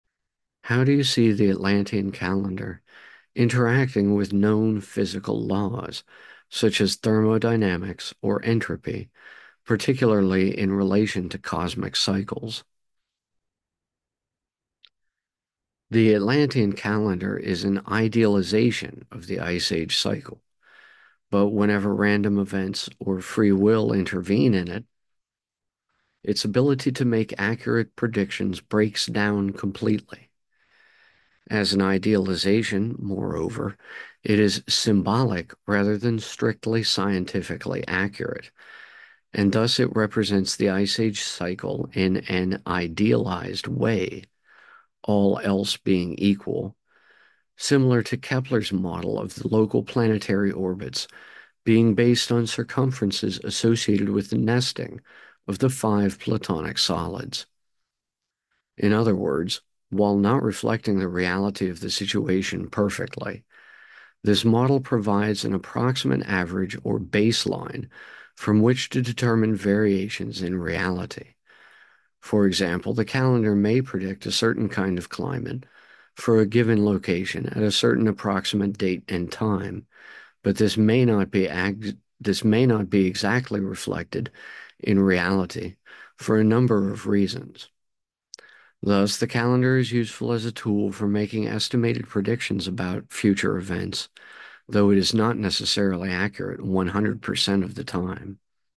On the Laws of Physics & the Atlantean Calendar
Q4.3. How do you see the Atlantean Calendar interacting with known physical laws, such as thermodynamics or entropy, particularly in relation to cosmic cycles?

The “Atlantean calendar” is an idealization of the “ice-age” cycle, but whenever random events or freewill intervene in it, its ability to make accurate predictions breaks down completely. As an “idealization,” moreover, it is symbolic rather than strictly scientifically accurate, and thus it represents the ice-age cycle in an “idealized” way - “all else being equal” - similar to Kepler’s model of the local planetary orbits being based on circumferences associated with the nesting of the 5 Platonic solids. In other words, while not reflecting the reality of the situation perfectly, this model provides an approximate average or “baseline” from which to determine variations in reality. For example, the calendar may predict a certain kind of climate for a given location at a certain approximate date and time, but this may not be exactly reflected in reality for a number of reasons. Thus, the calendar is useful as a tool for making estimated predictions about future events, though it is not necessarily accurate 100% of the time.